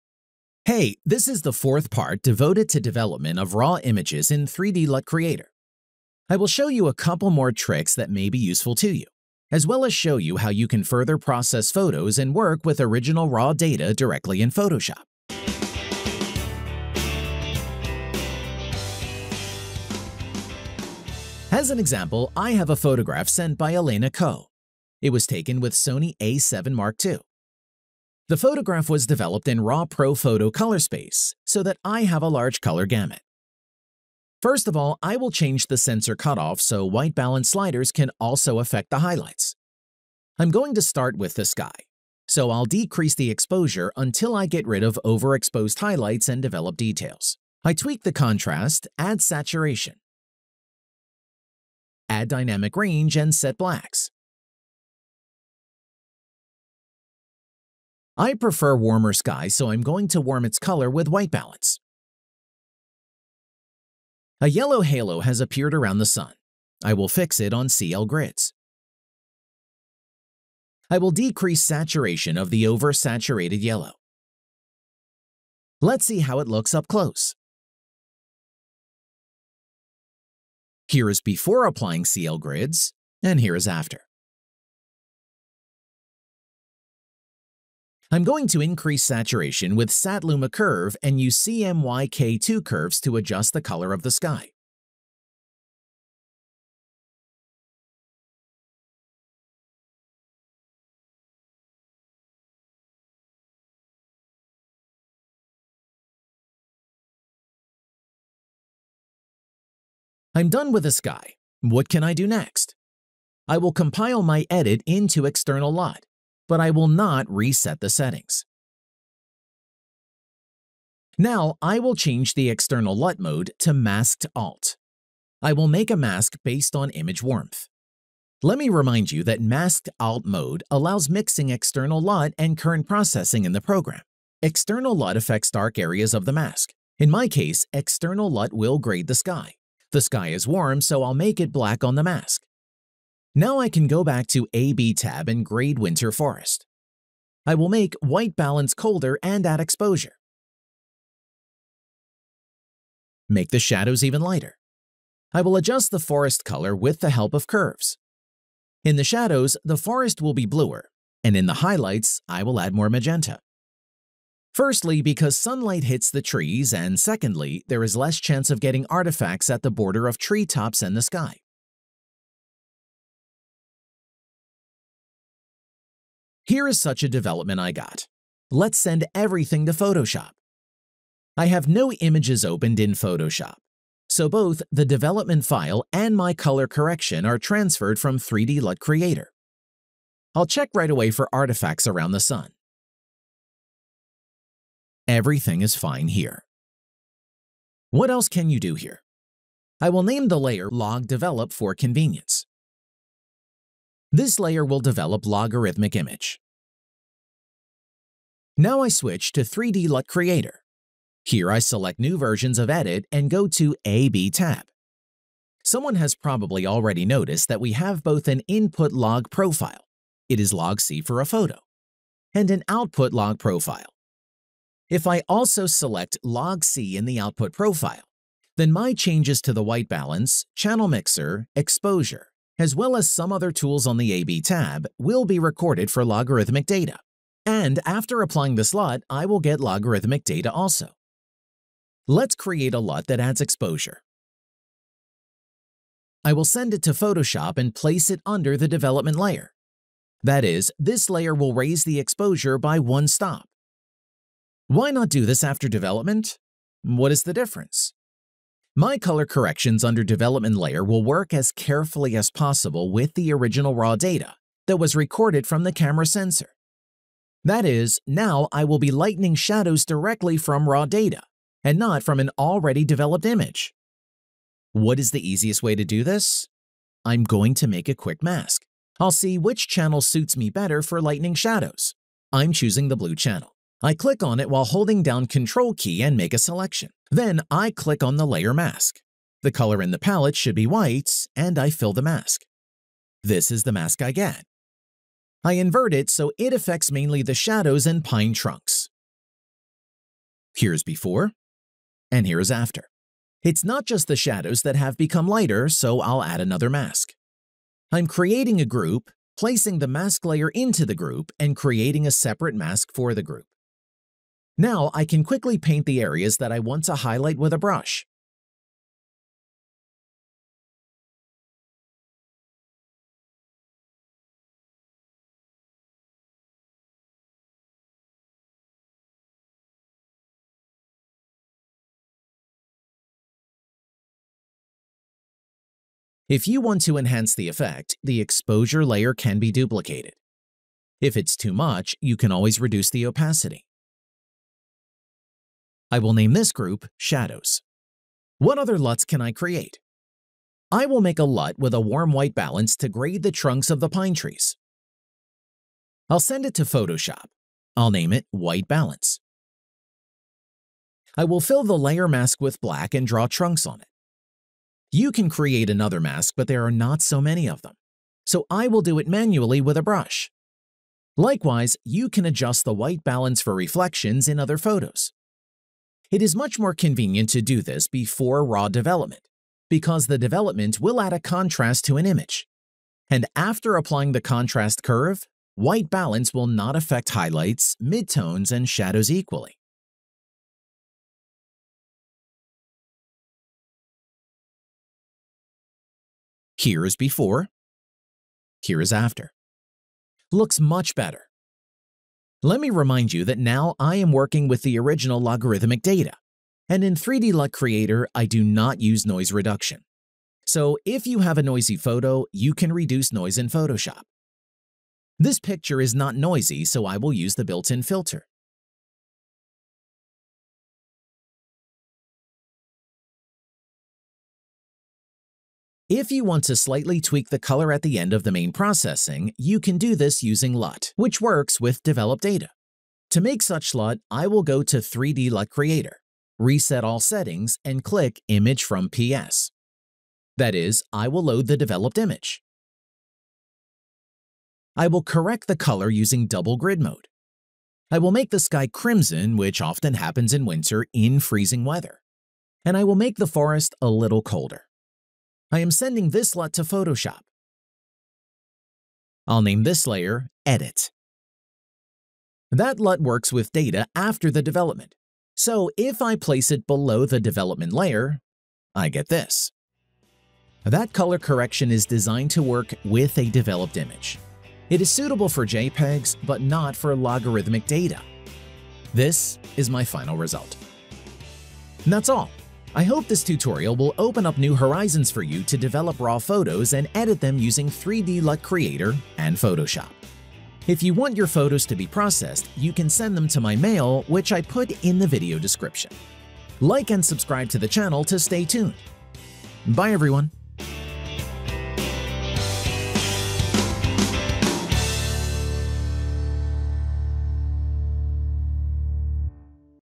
How to develop RAW files in 3D LUT Creator, Part 4
Hey! This is the fourth part, devoted to development of RAW images in 3D LUT Creator. I will show you a couple more tricks that may be useful to you, as well as show how you can further process photos and work with original RAW data directly in Photoshop.

As an example, I have a photograph sent by Elena Ko. It was taken with Sony A7 Mark 2. The photograph was developed in RAW ProPhoto color space so that I have a large color Gamut. First of all, I will change the sensor cutoff so white balance sliders can also affect the highlights. I'm going to start with the sky, so I'll decrease the exposure until I get rid of overexposed highlights and develop details. I tweak the contrast, add saturation, adjust dynamic range and set Blacks. I prefer warmer sky, so I'm going to warm its color with white balance. I’m going to increase saturation with Sat/Luma curve and use CMYK 2 curves to adjust the color of the sky.

I'm done with the sky, what can I do next? I will compile my edit into external LUT, but I will not reset the settings. Now I will change the external LUT mode to Masked Alt. I will make a mask based on image warmth. Now I can go back to A/B tab and grade winter forest. I will make white balance colder and add exposure, make the shadows even lighter. I will adjust the forest color with the help of curves.  Here is such a development I got, let’s send everything to Photoshop.

What else can you do here? I will name the layer with LUT “LOG develop” for convenience. This layer will develop logarithmic image. Now I switch to 3D LUT Creator, here I select new version of edit and go to A/B tab. Someone has probably already noticed that we have both an input Log-profile (it is LogC for a photo), and an output Log-profile. If I also select LogC in the output profile, then my changes to the white balance, channel mixer, exposure, as well as some other tools on the AB tab, will be recorded for logarithmic data and after applying this LUT I will get logarithmic data also. Let’s create a LUT that adds exposure! My color corrections under development layer will work as carefully as possible with the original RAW data that was recorded from the camera's sensor. That is, now I will be lightening shadows directly from RAW data, and not from an already developed image! What is the easiest way to do this? I'm going to make a quick mask.

It's not just the shadows that have become lighter, so I'll add another mask. I’m creating a group, placing the mask layer into the group and creating a separate mask for the group. Now I can quickly paint the areas that I want to highlight with a brush. If you want to enhance the effect, the exposure layer can be duplicated.
What other LUTs can I create? I will make a LUT with a warm white balance to grade the trunks of the pine trees. I'll send it to Photoshop, I'll name it “White Balance”. I will fill the layer mask with black and draw trunks on it. You can create another mask, but there are not so many of them, so I will do it manually with a brush. Likewise, you can adjust the white balance for reflections in other photos. It is much more convenient to do this before RAW development, because the development will add a contrast to an image, and after applying the contrast curve, white balance will not affects highlights, midtones and shadows equally. Here is before, here is after! Looks much better! Let me remind you that now I am working with the original logarithmic data, and in 3D LUT Creator I do not use noise reduction, so if you have a noisy photo, you can reduce noise in Photoshop. This picture is not noisy, so I will use the built-in filter.

If you want to slightly tweak the color at the end of the main processing, you can do this using LUT, which works with developed data. To make such LUT, I will go to 3D LUT Creator, reset all settings and click Image from PS, that is, I will load the developed image. I will correct the color using double grid mode. I will make the sky crimson, which often happens in winter in freezing weather, and I will make the forest a little colder. I am sending this LUT to Photoshop. This is my final result!

That's all! I hope this tutorial will open up new horizons for you to develop RAW photos and edit them using 3D LUT Creator and Photoshop.
If you want your photos to be processed, you can send them to my mail, which I put in the video description.
Bye everyone!